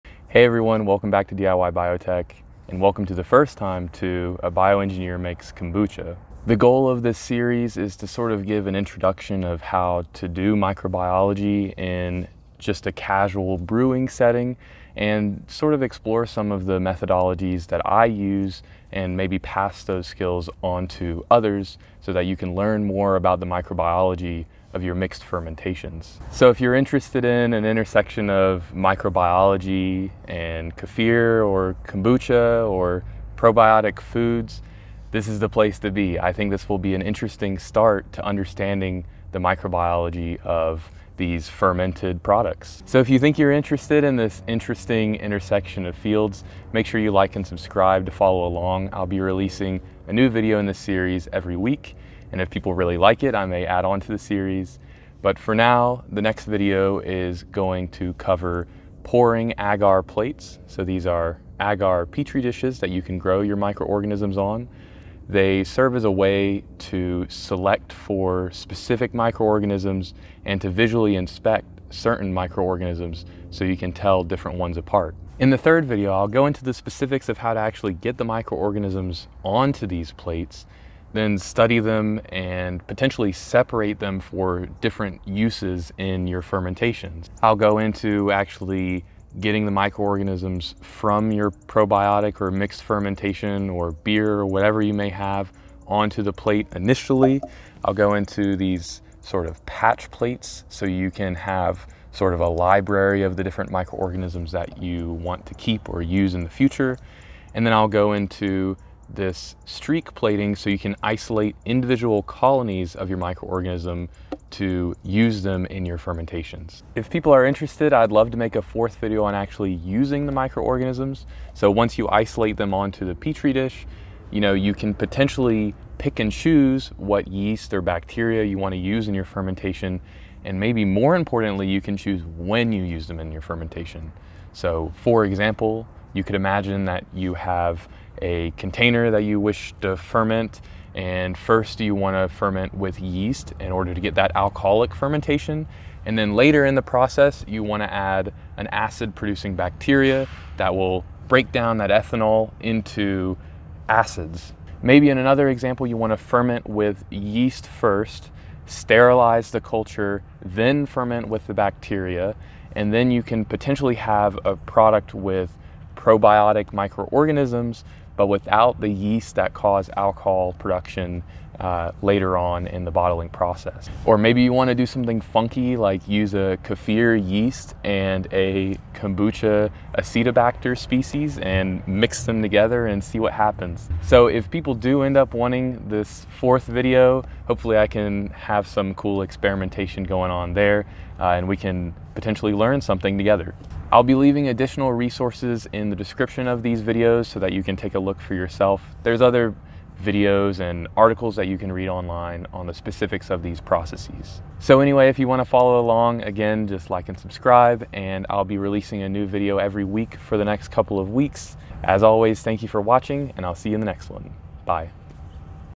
EP. 1: Introduction | A Bioengineer Makes Kombucha
First introduction episode of "A Bioengineer Makes Kombucha"!
Here I go over the goals of the series and what will be covered including:
patch plating, colony isolation, colony selection, preparing agar plates, and more!

Let me know if you have recommendations for future videos in the comments below!